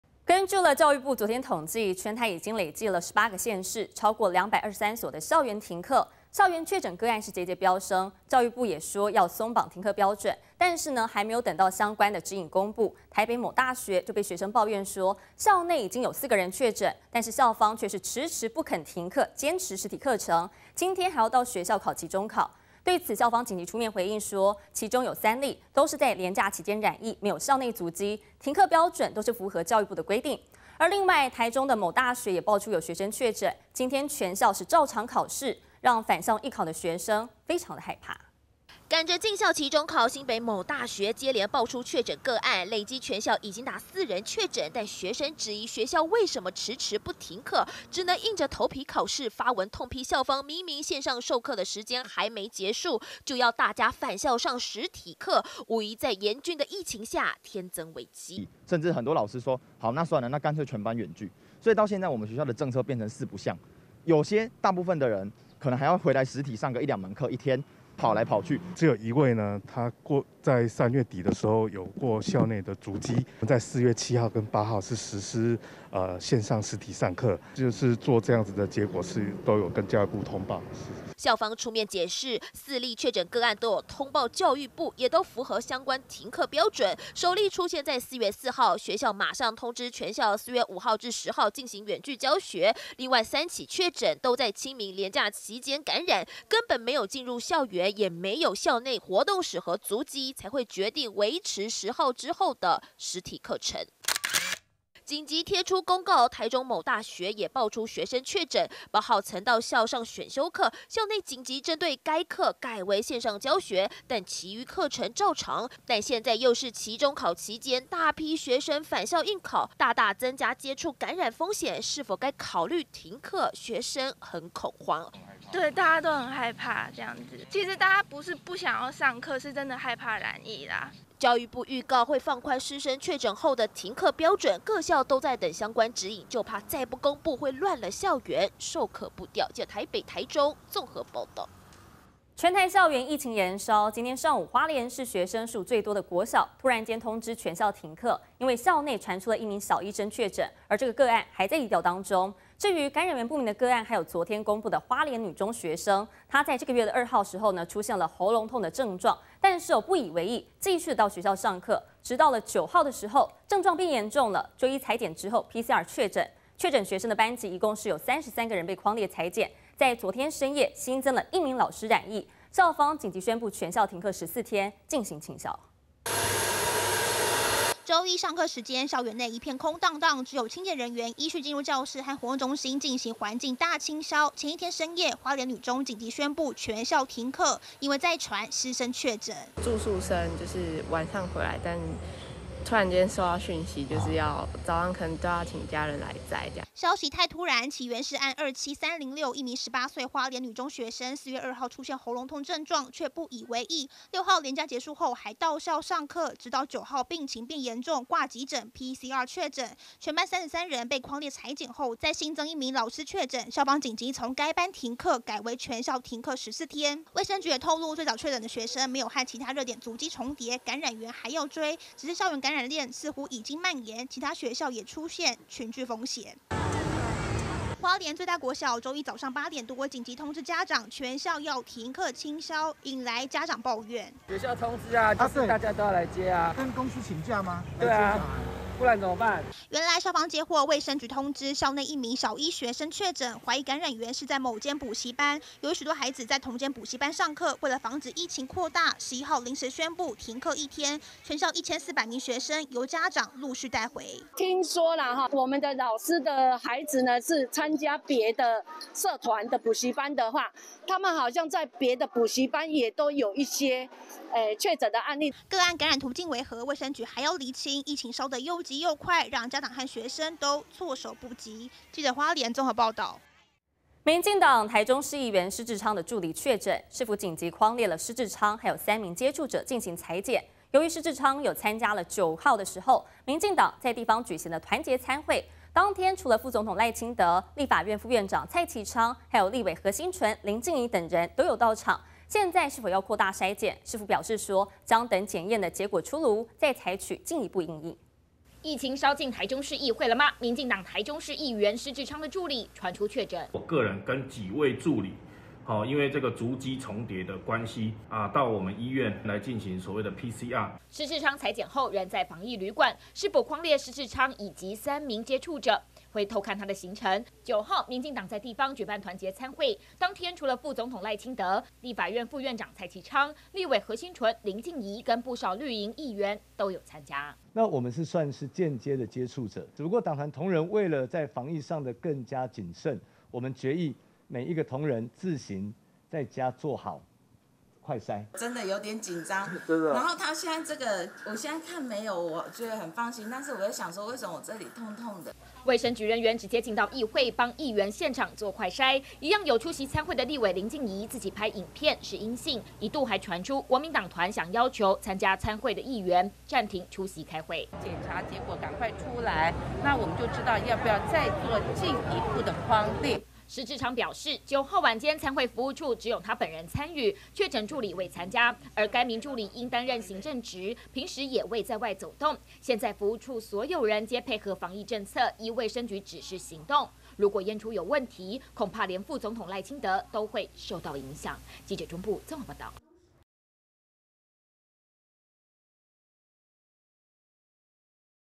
校內有確診 停班課標準不一學生怨【重點新聞】-20220411
(00:00)累計4確診 新北某大學恢復實體課學生怨
(02:22)喉嚨痛不以為意 花蓮女中學生7天後確診
(04:34)助理確診！市議員施志昌曾參加綠營餐會